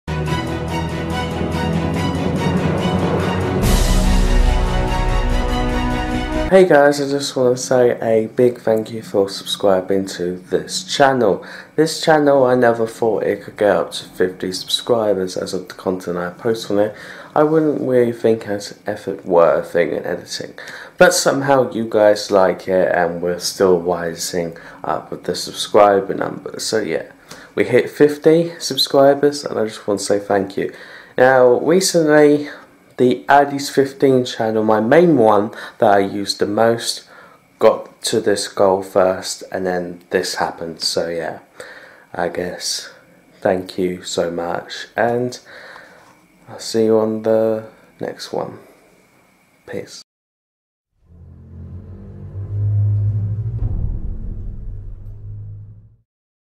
WE HIT 50 SUBS (: !!!! (Thank you)
Hey Guys so just a little video to tell you thank you for subing to this channel ... everyday i am growing this channel with more subs (: !!!

Adis 15 first hit subs and now its adis games turn (: !!!

Thanks AH (: